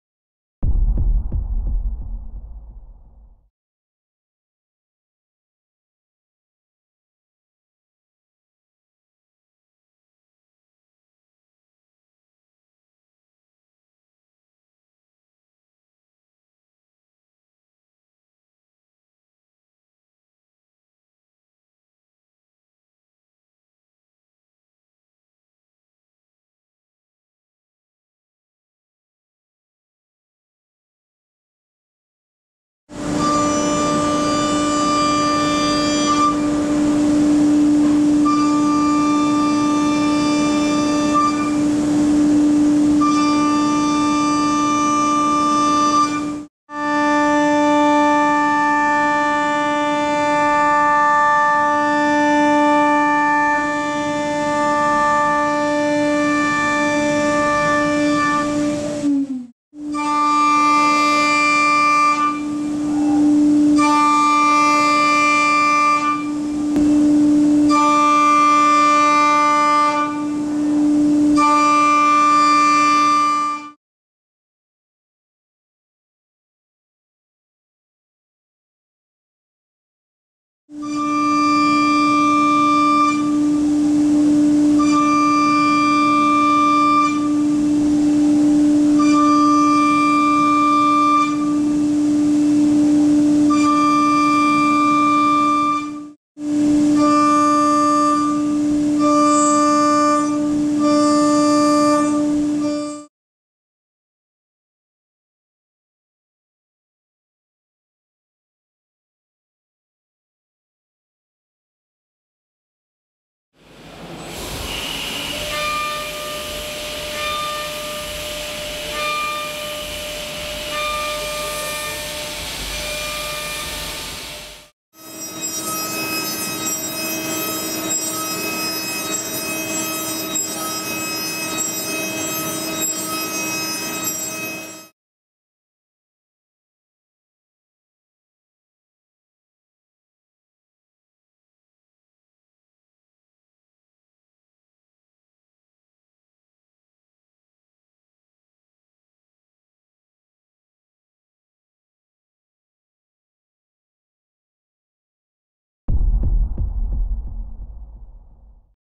WALTER MC275 - фрезеруем жаропрочные сплавы керамической фрезой
Новая линейка фрез MC275 от Walter - керамические фрезы, предназначенные для работы по сложным в обработке жаропрочным сплавам. Скорость резания 600 м/мин по Инконелю? Невероятно? Нет, с керамическими фрезами MC275 это вполне возможно!